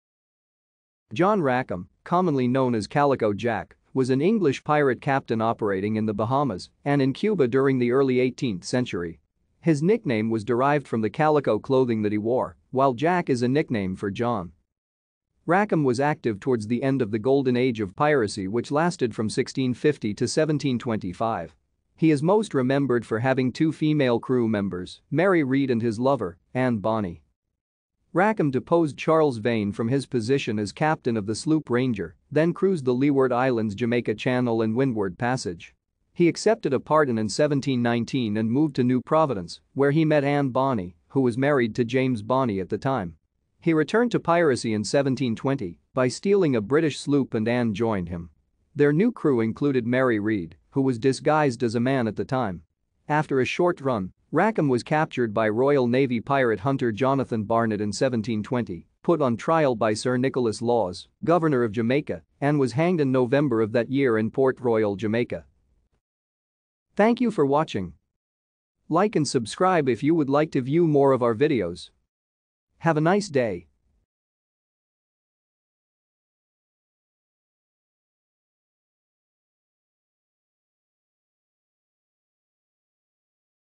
Calico Jack
John Rackham, commonly known as Calico Jack, was an English pirate captain operating in the Bahamas and in Cuba during the early 18th century. His nickname was derived from the calico clothing that he wore, while Jack is a nickname for "John".